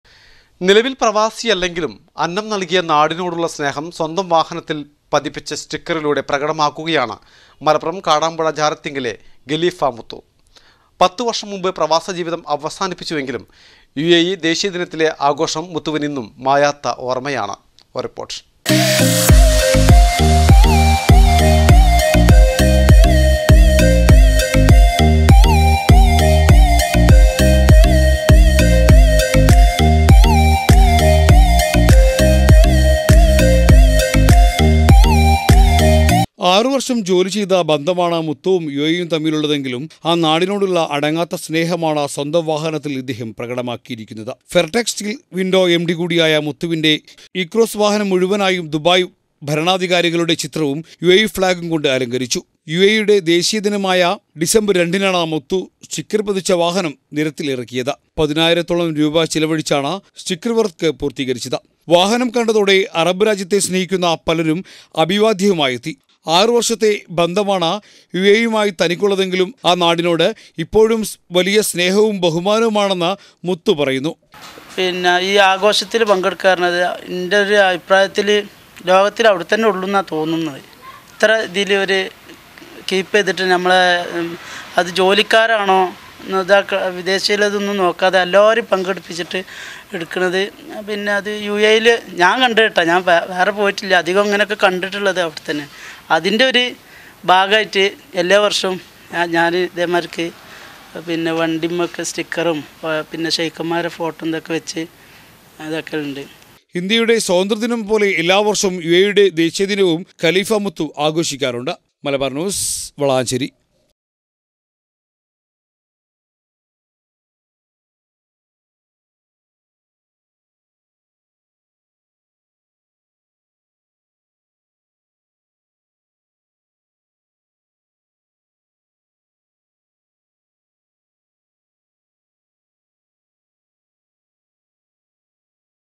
അന്നം നല്‍കിയ നാടിനോടുള്ള സ്നേഹം സ്വന്തം വാഹനത്തില്‍ പതിച്ച് ഖലീഫ മുത്തു - uae independence day
നിലവില്‍ പ്രവാസിയല്ലെങ്കിലും അന്നം നല്‍കിയ നാടിനോടുള്ള
സ്നേഹം സ്വന്തം വാഹനത്തില്‍ പതിച്ച സ്റ്റിക്കറിലൂടെ പ്രകടമാക്കു
കയാണ് മലപ്പുറം കാടാമ്പുഴ ജാറത്തിങ്ങല്ലിലെ ഖലീഫ മുത്തു. പത്ത്
വര്‍ഷം മുമ്പ് പ്രവാസ ജീവിതം അവസാനിപ്പിച്ചുവെങ്കിലും യു.എ.ഇ
ദേശീയ ദിനത്തിലെ  ആഘോഷം മുത്തുവിന് ഇന്നും മായാത്ത
ഓര്‍മ്മയാണ്.